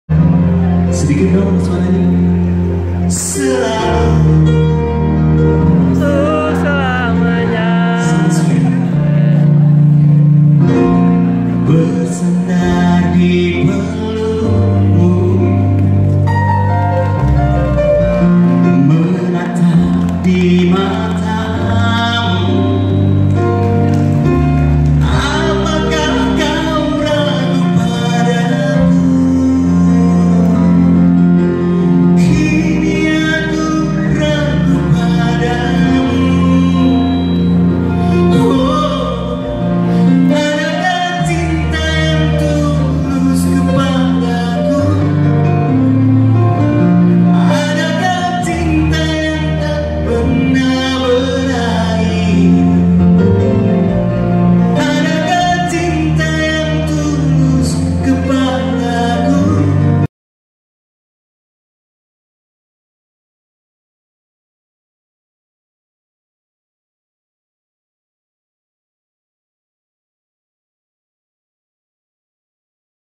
FATUR JAVA JIVE (ORANG BANJAR) DI ACARA IDI (IKATAN DOKTER INDONESIA) WILAYAH KALSEL
Sebuah lagu persembahan Fatur Java Jive di acara malam gathering keluarga dokter kalsel 2016